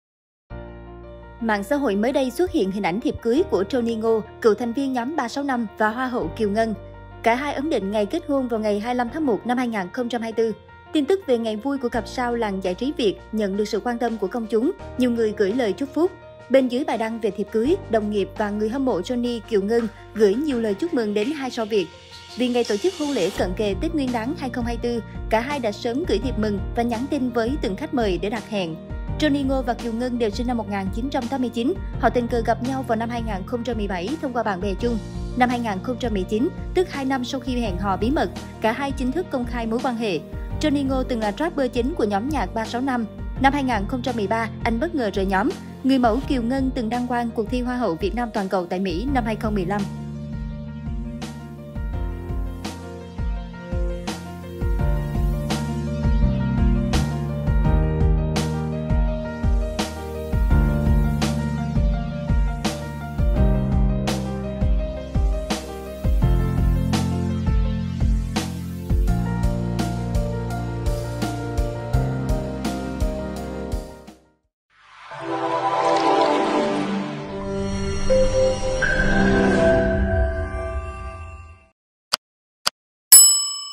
Vbiz thêm tin vui: cặp đôi ca sĩ - Hoa hậu kết hôn | Vén màn showbiz
Mạng xã hội mới đây xuất hiện hình ảnh thiệp cưới của Tronie Ngô - cựu thành viên nhóm 365 và Hoa hậu Kiều Ngân

* Thực hiện: LYLY - TATA